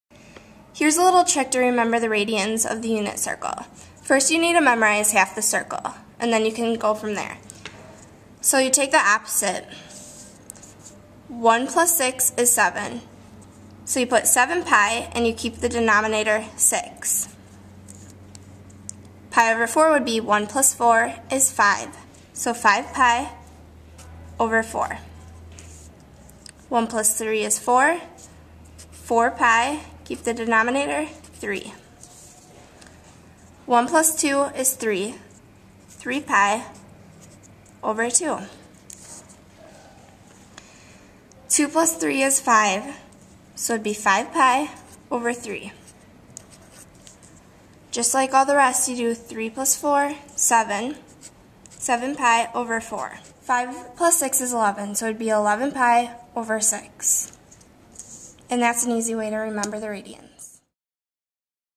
Radians Around the Unit Circle
This is a video one of my students made regarding a strategy she came up with to help remember the radian measures around the unit circle. I think it's a pretty neat tool. Enjoy!